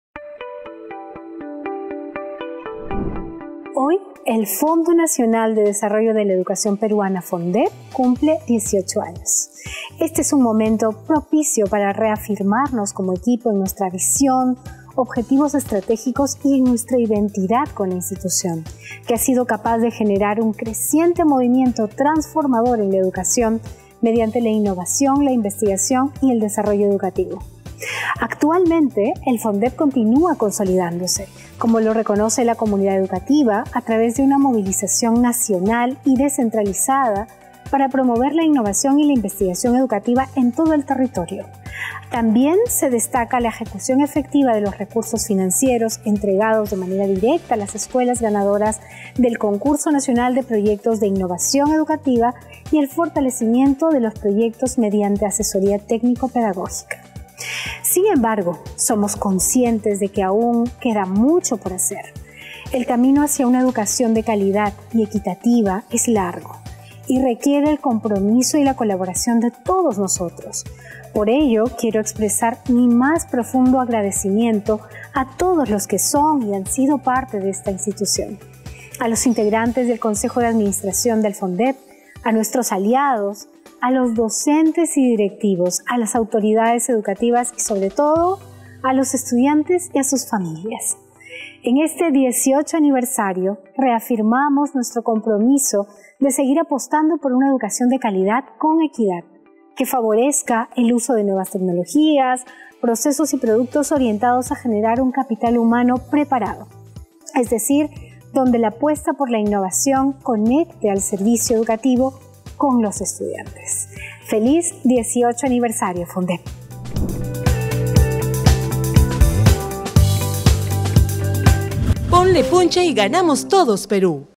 ¡Hoy celebramos nuestro aniversario!
🎉En estos 18 años de compromiso y excelencia.
Agradecemos a todos los que han sido parte de este camino hacia la transformación educativa.🎊
¡Sigamos construyendo juntos un futuro brillante para la educación en nuestro país! AniversarioFondep EducaciónInnovadora"